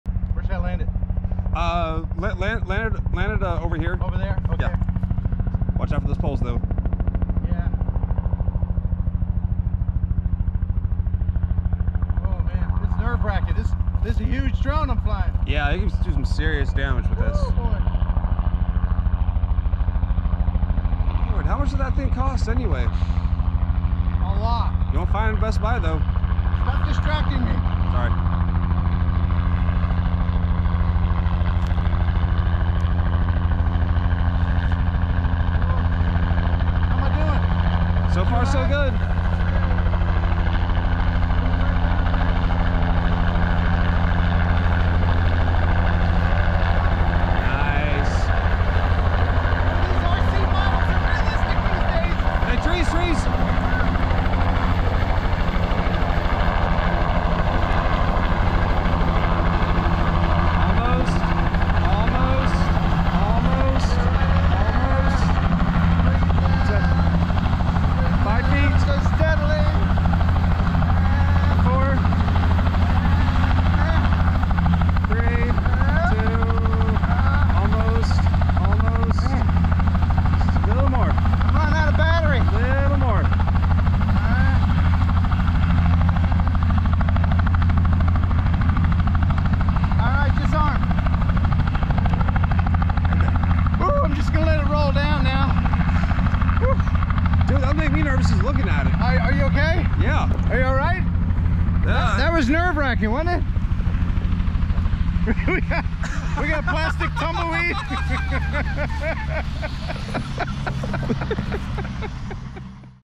Flying the World's LARGEST RC Helicopter
Ken landed an enormous 1:1 scale RC Helicopter!

Please consider supporting this channel with a Membership.

Links to stuff in this video:
DJI Goggles 2